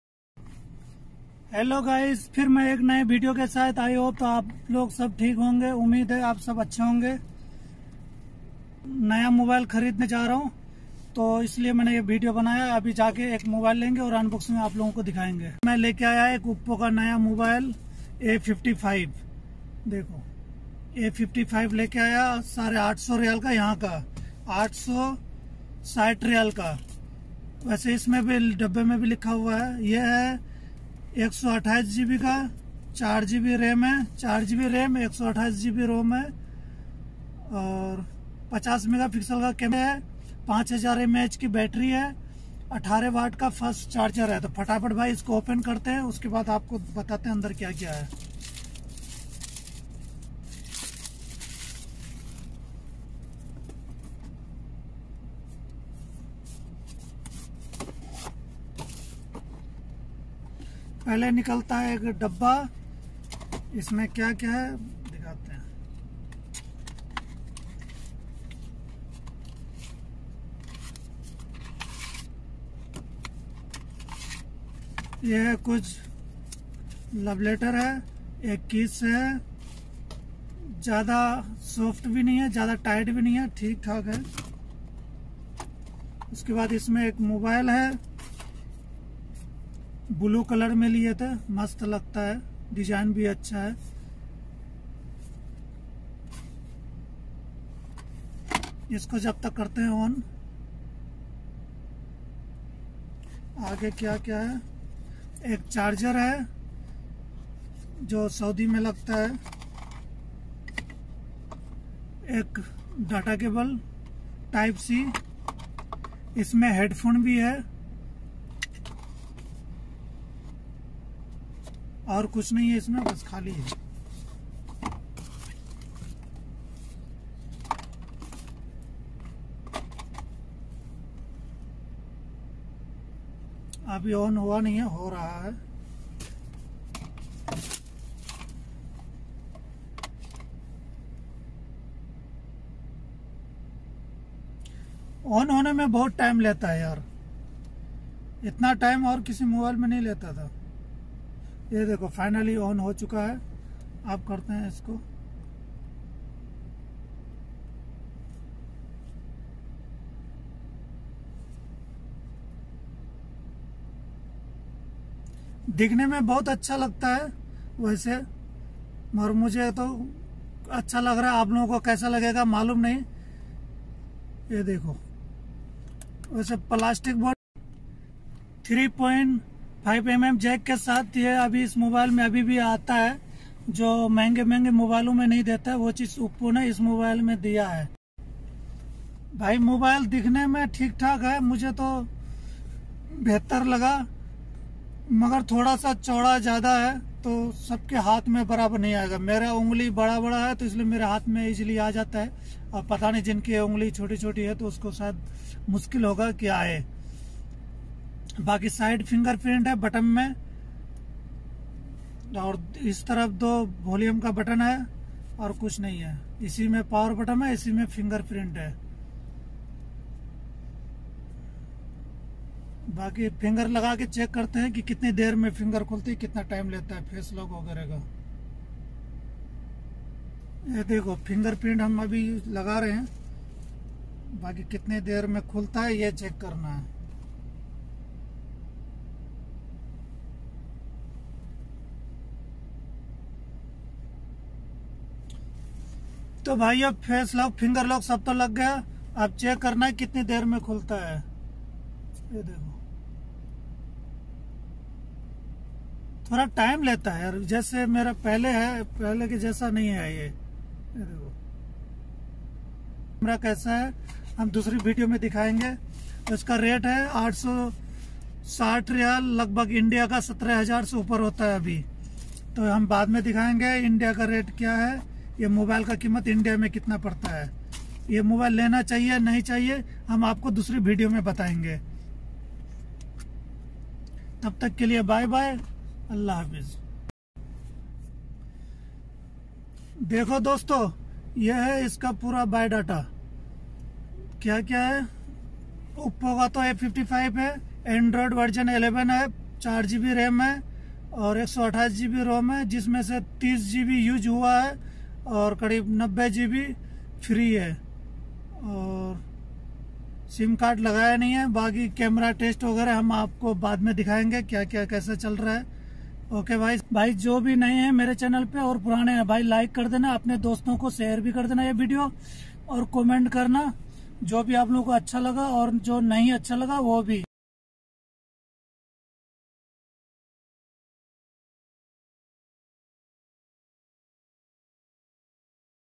oppo a55 unboxing ! हिन्दी ! oppo phone unboxing
Kya jisme 50MP men camera hai
16 MP Camara hai 5000 mah battery hai 18 wat fust charger hai
TFT LCD hai
Iske sath silicon back cover hai